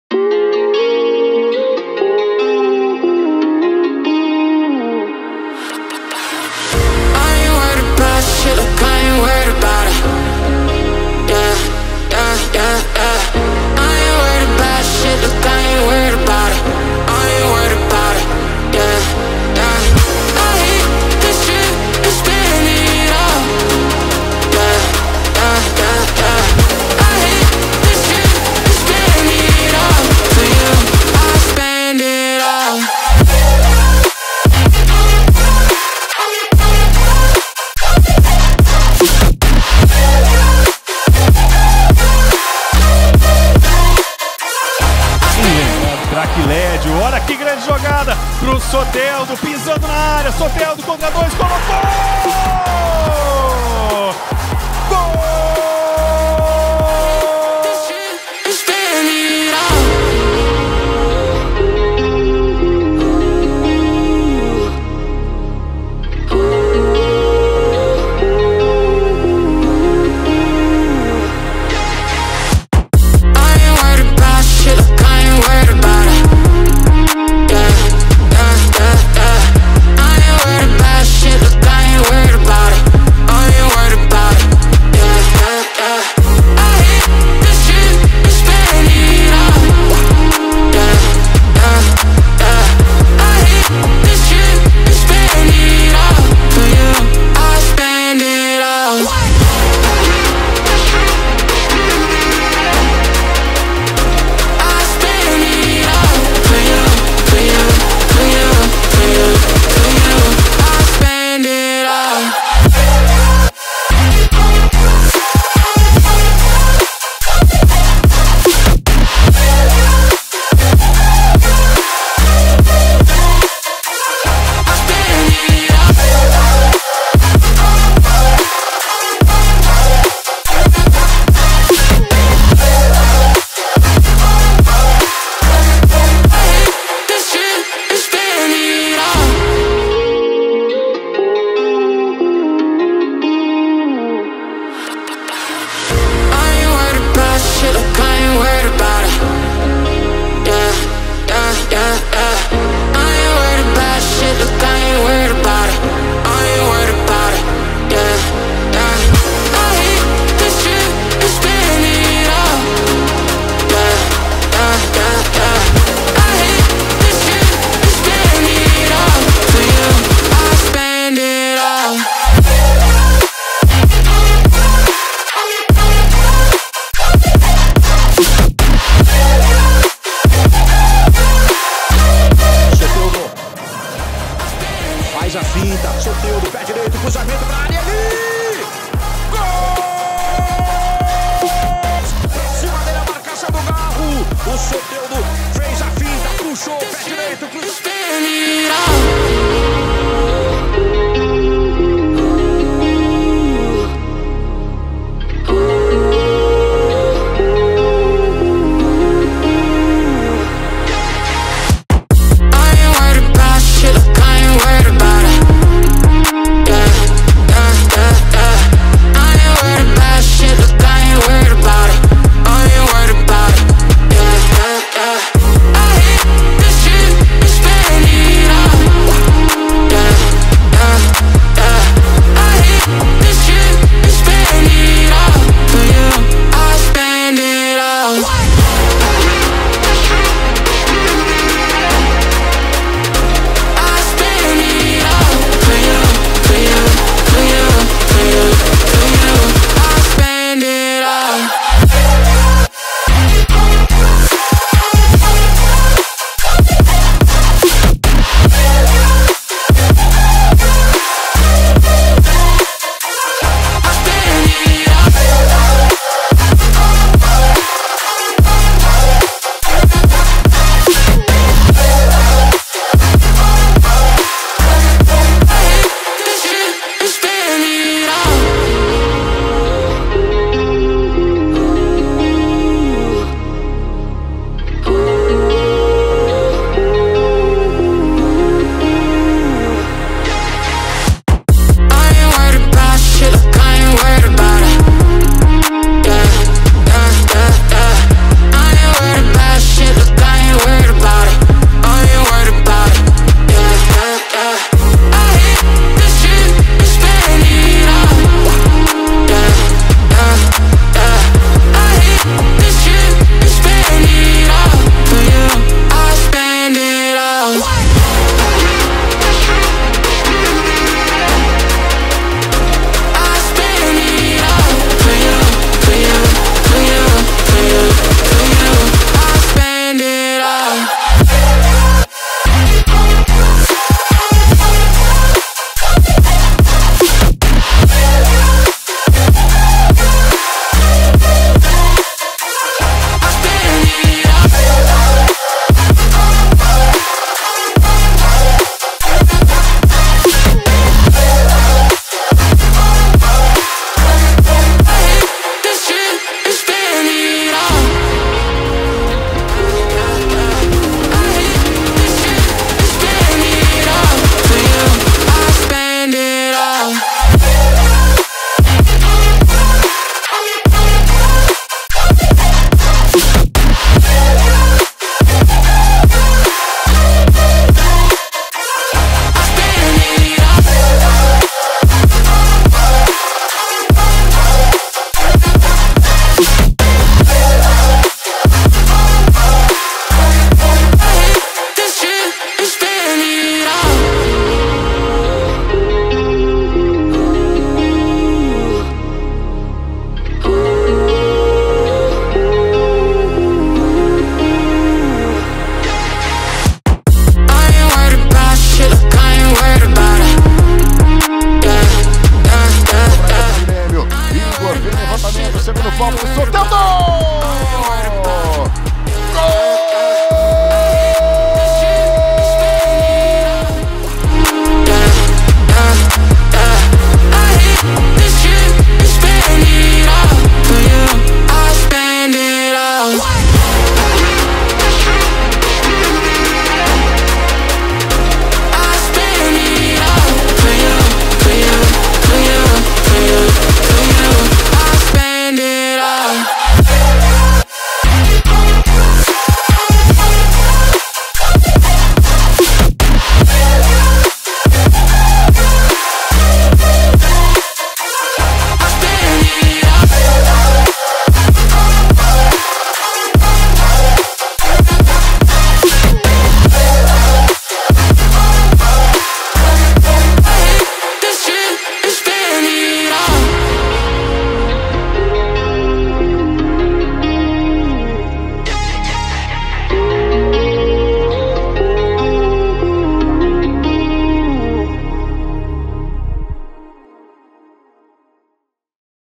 Yeferson Soteldo 2024 - Sublime Skills, Assists & Goals - Grêmio | HD
Soteldinho - Dribles, Assistências & Gols pelo Imortal 2024